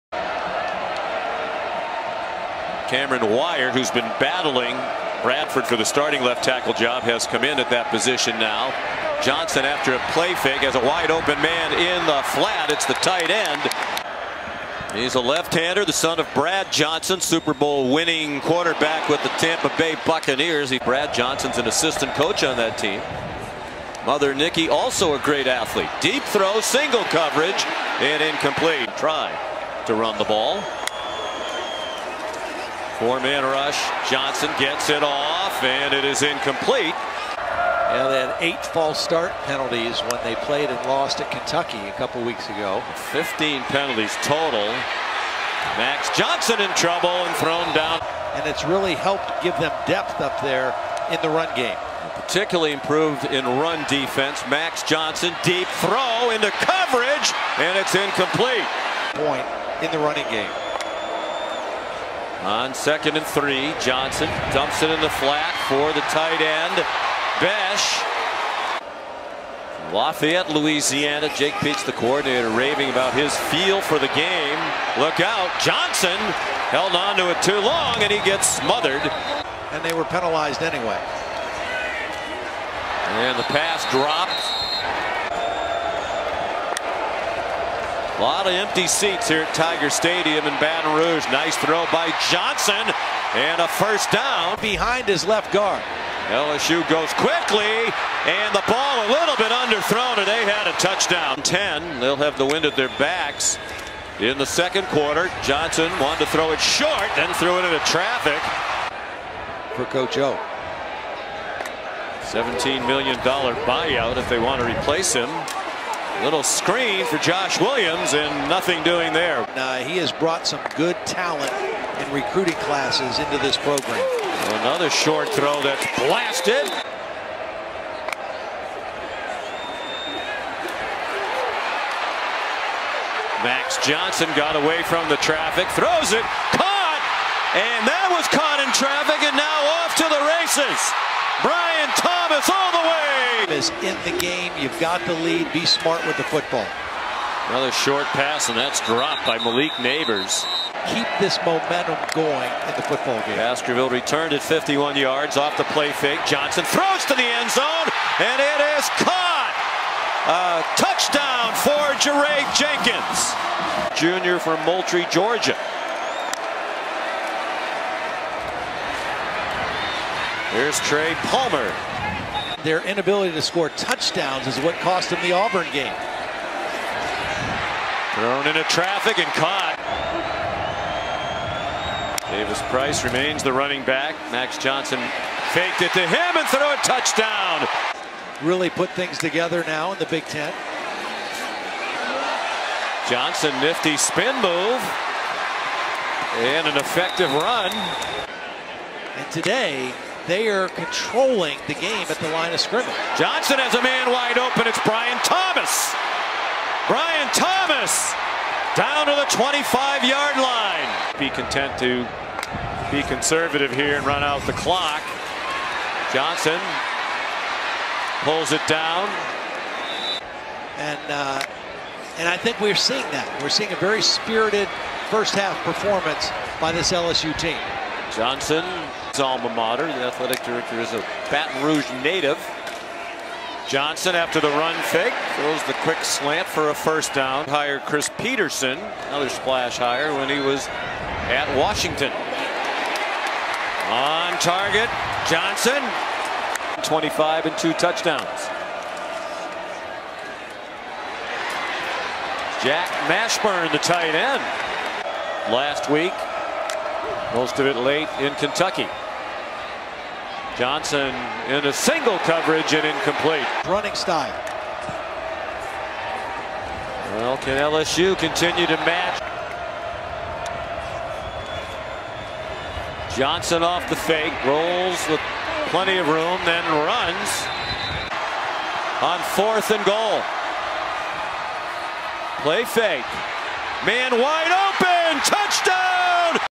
Max Johnson vs Florida (2021)
All passing & rushing attempted by LSU quarterback Max Johnson in the 2021 season game against Florida.

NOTE - Max Johnson has since transferred to Texas A&M.

Game Stats;
Passing - 14/24, 133 yards, 3 TDs

If you notice an error with a cutup, have a suggestion for a game you'd like to see cut, or just want to see one pointless Tweet a month, my Twitter is